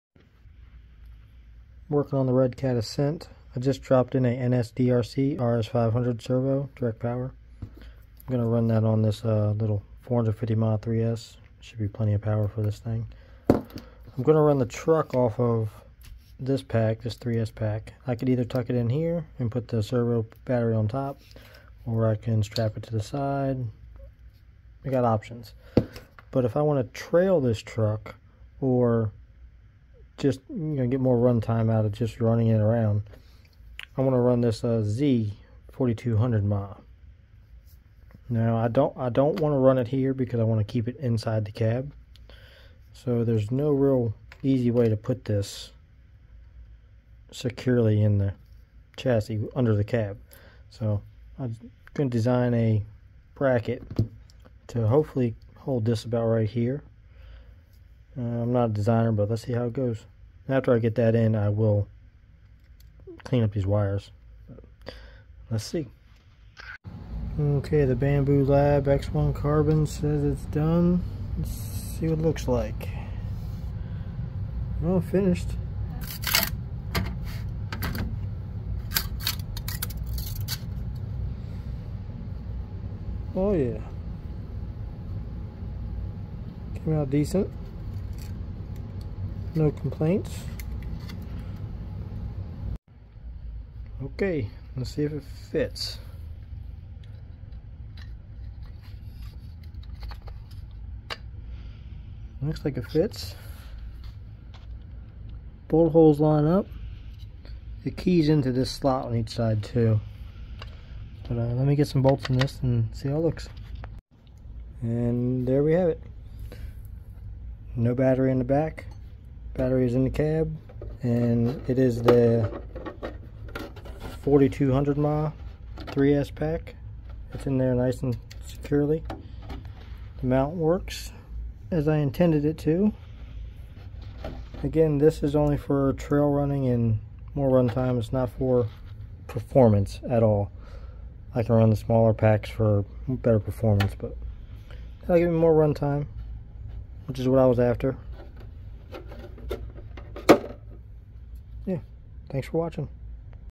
newbie designs battery tray
Nothing fancy, just a video showing a battery tray design I came up with for my redcat ascent.

printed on a Bambu Labs x1 carbon in a pla cf filament.